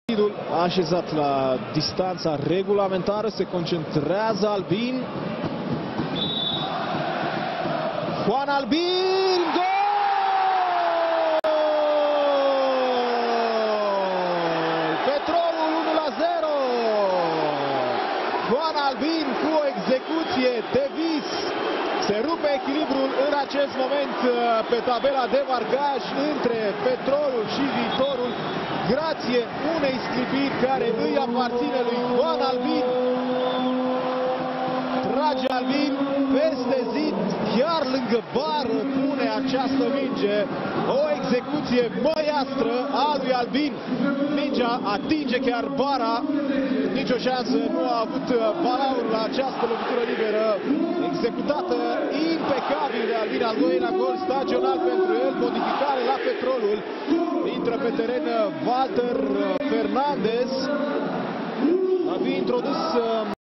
Juan Ángel Albín.. Petrolul Ploiesti vs Viitorul (3-0)
GOOOOOOL PETROLUL PLOIESTI ! A marcat pentru Petrolul... Jucatorul cu numarul #11 JUAN ALBIN !